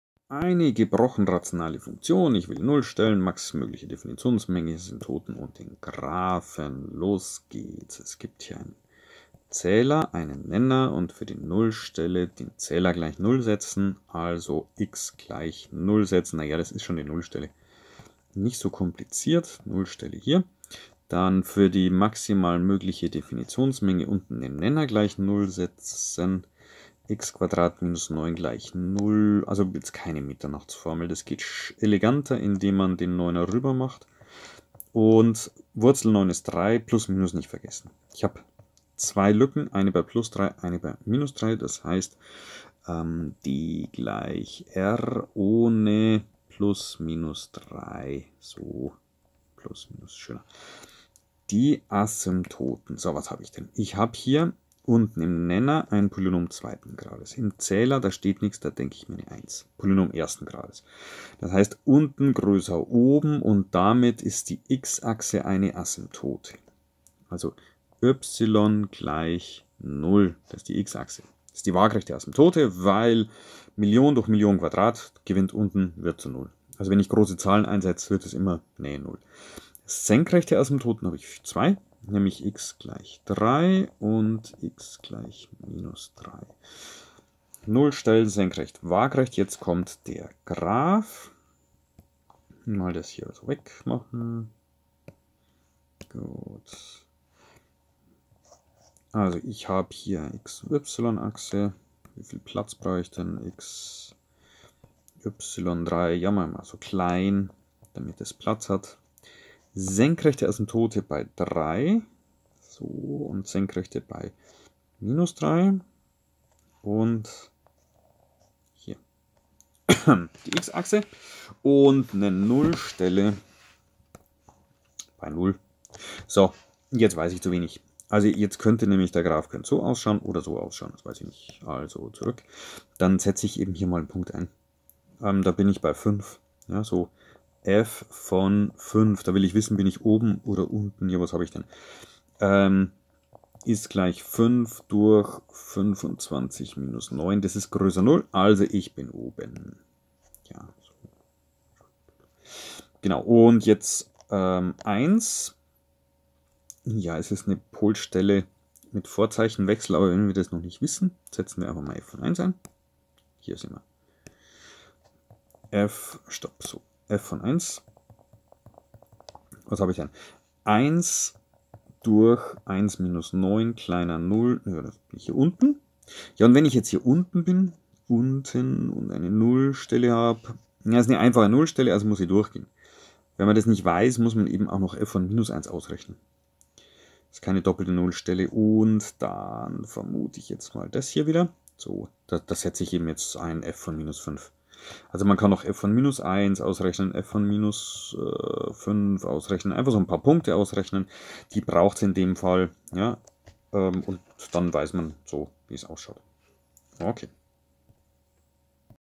M10 Eine gebrochen rationale Funktion untersuchen (3)
Nullstellen, maximal mögliche Definitionsmenge, Asymptote und Graphen skizzieren,